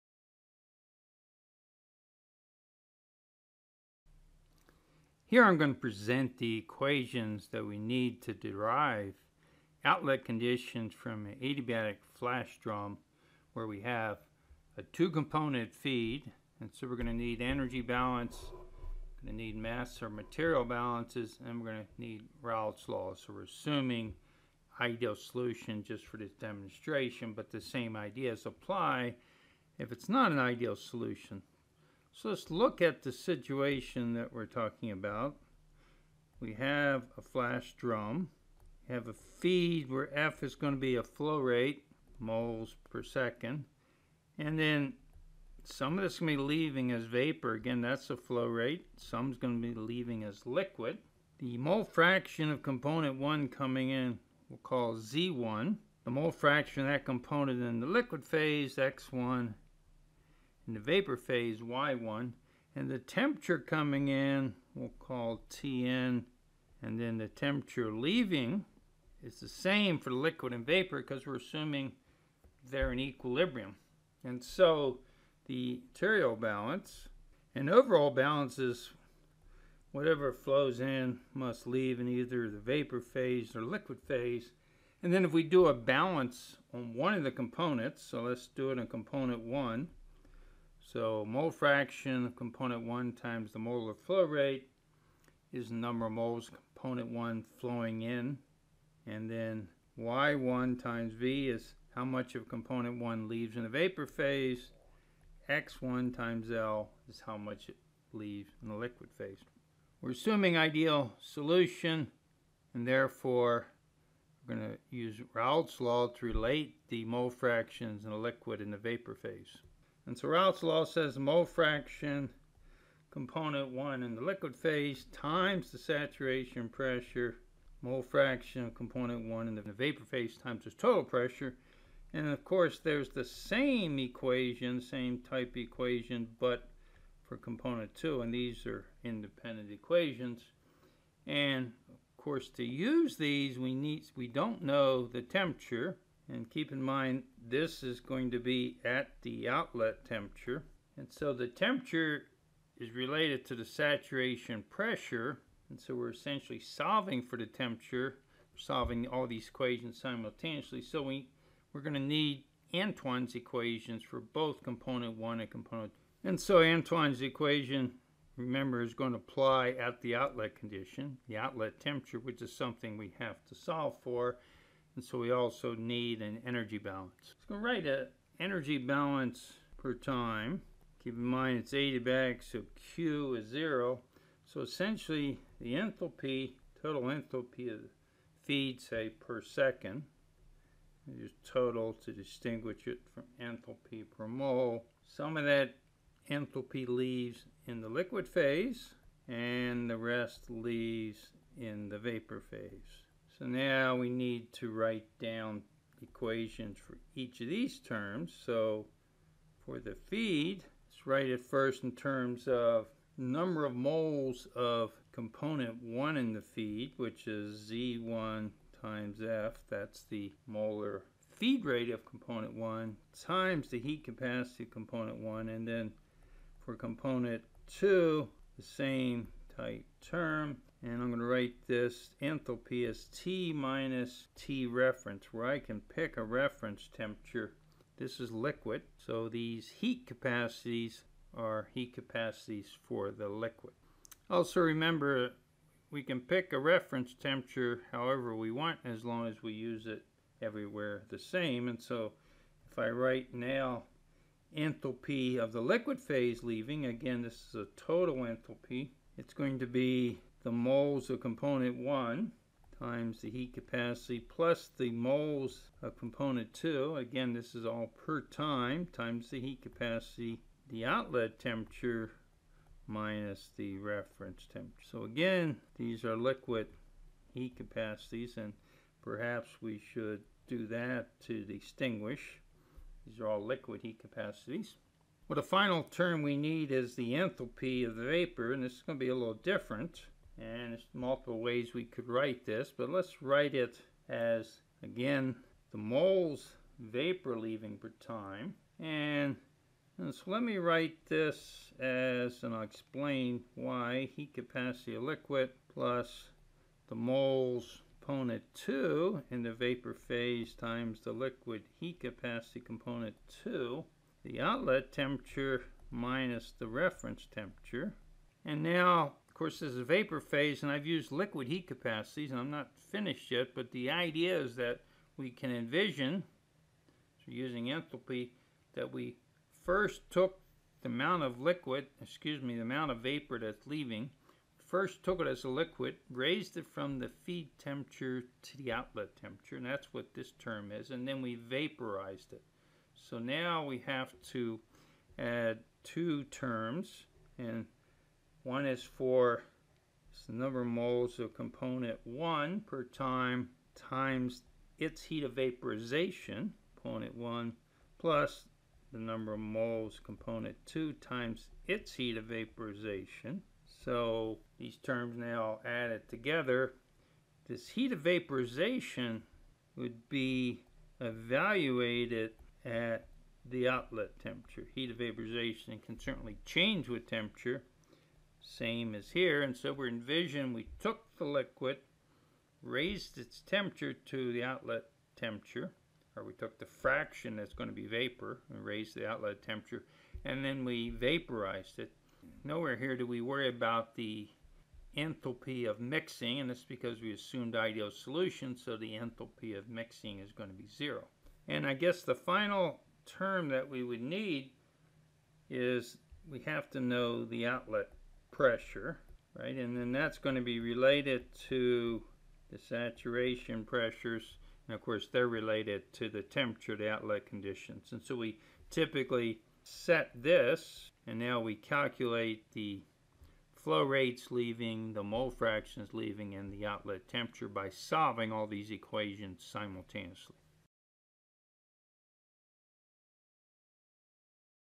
Balances for an Adiabatic Flash Drum
Organized by textbook Presents the material balances, energy balance, and Raoult's law for an adiabatic flash drum. The feed is an ideal binary liquid. Made by faculty at the University of Colorado Boulder, Department of Chemical & Biological Engineering.